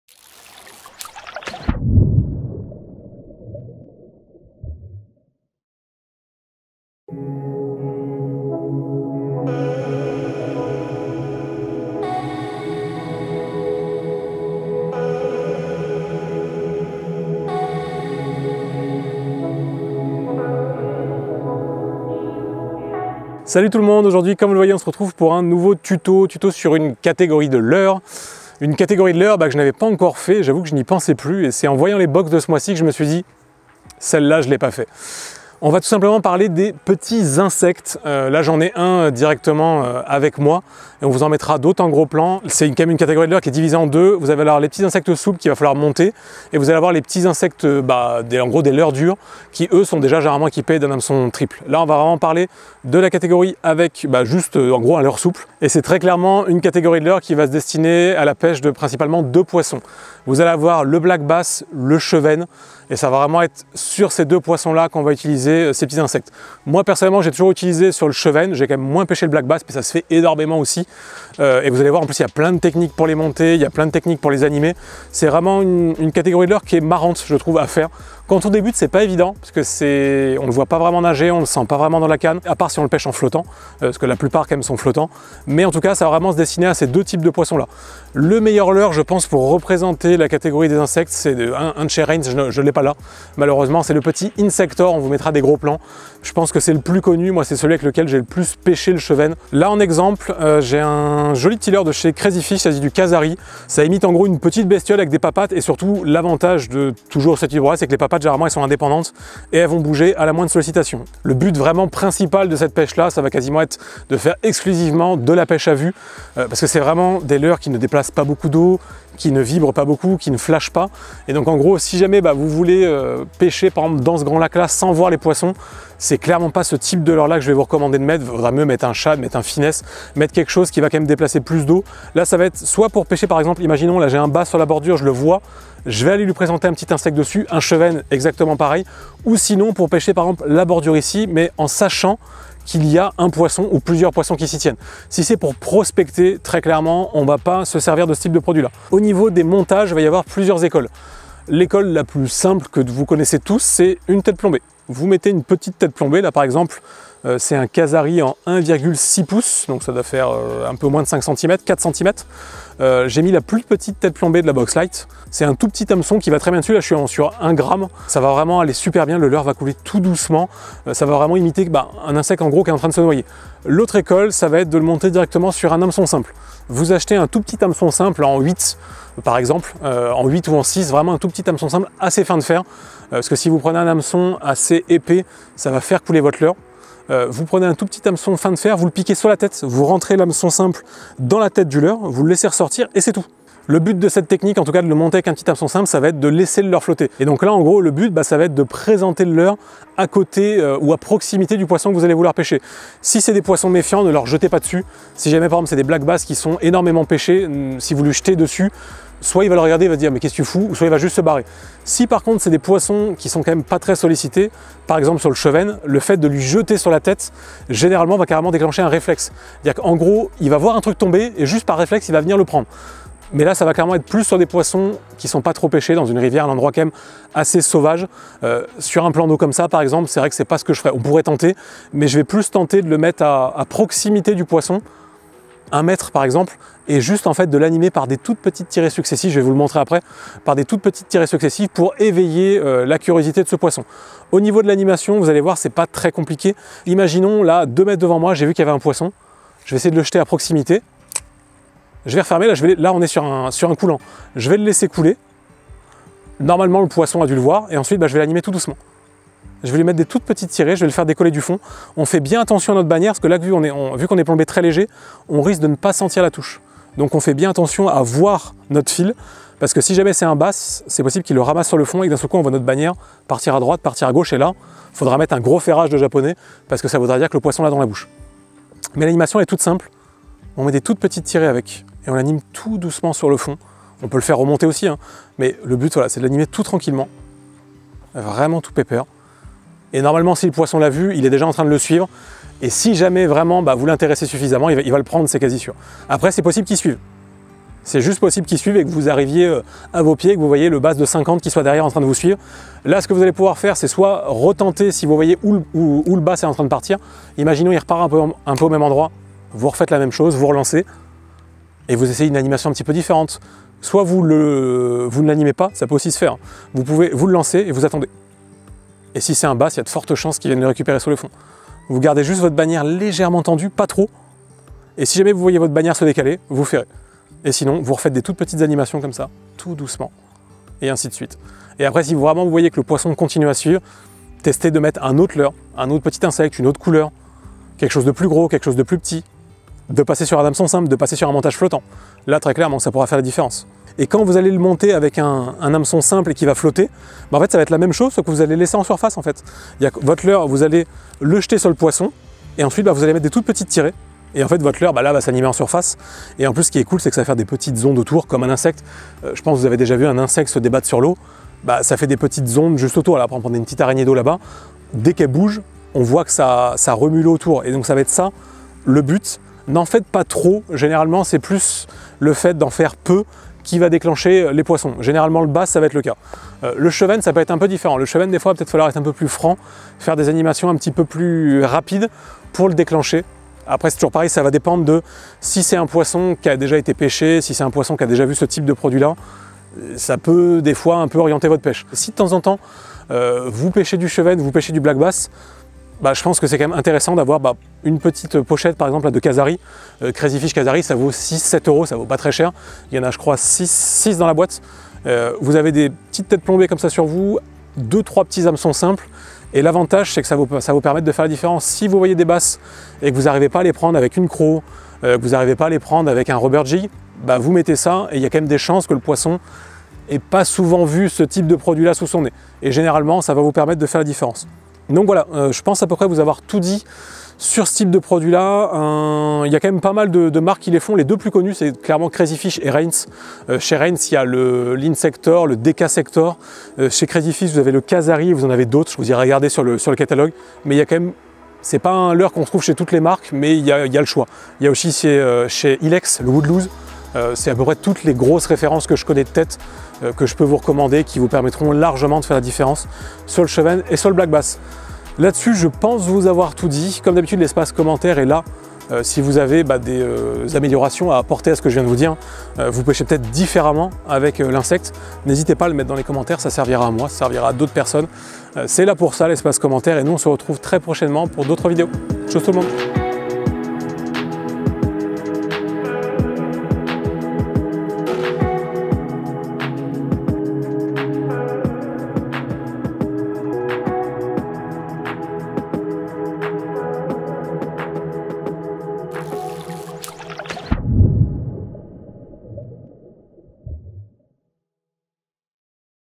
Wiki pêche - Tout SAVOIR sur les INSECTES souples
Dans ce nouveau Tuto pêche, je vais tenter de t'expliquer tout ce que je sais sur un leurre en particulier, les petits insectes souples. Un type de leurre qui peut s'avérer redoutable quand les chevesnes ou les black bass sont méfiants.